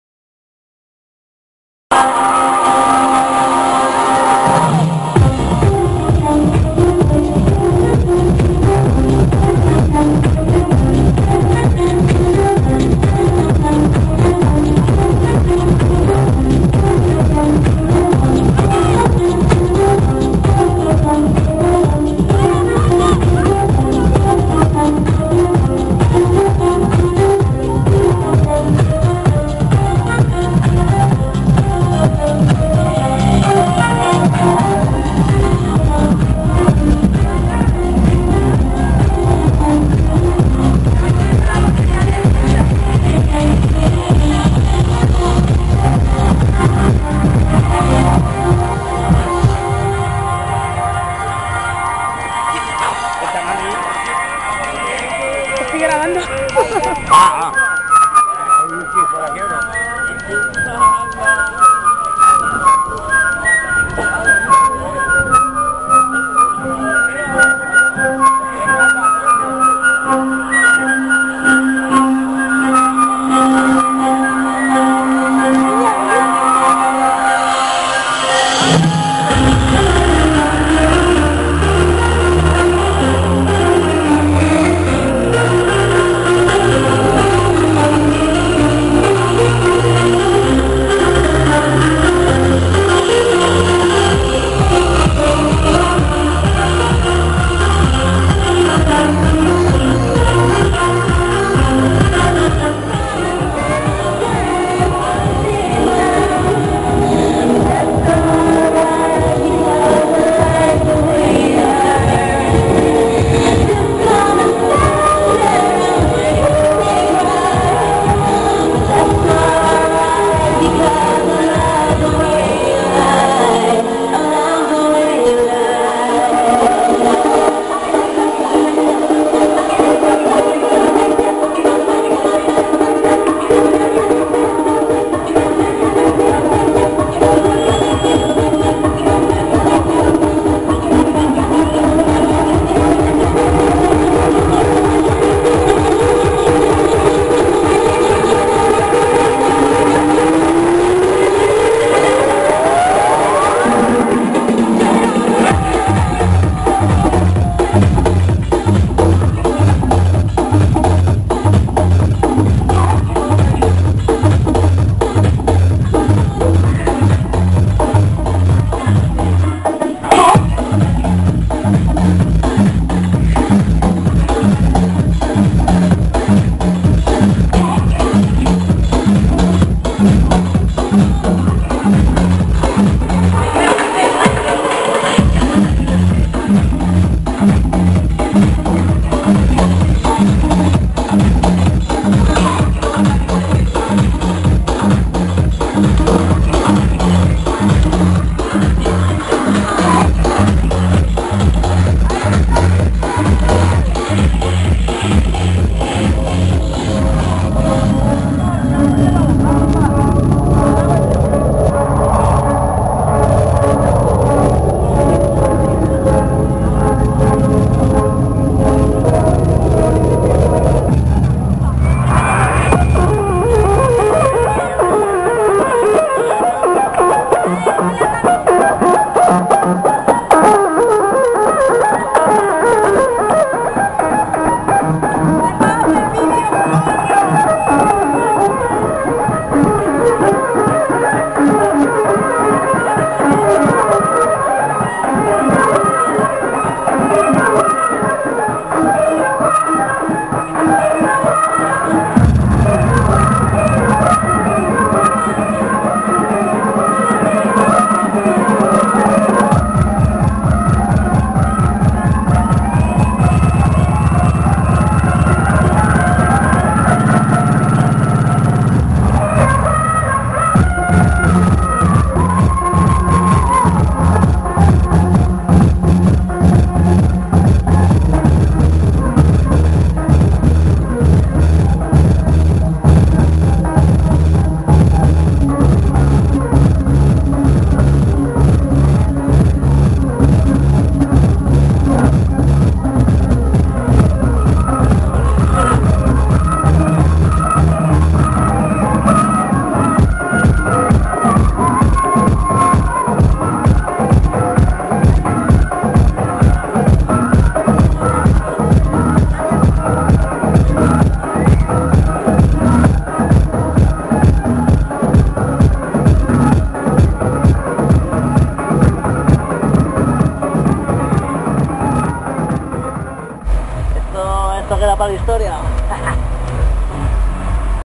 FmF Group Rave en San Isidro.
Para un buen cierre de las fiestas patronales de San Bartolomé en Belmonte. FmF Group a subido a San Isidro a seguir animando la mañana¡¡¡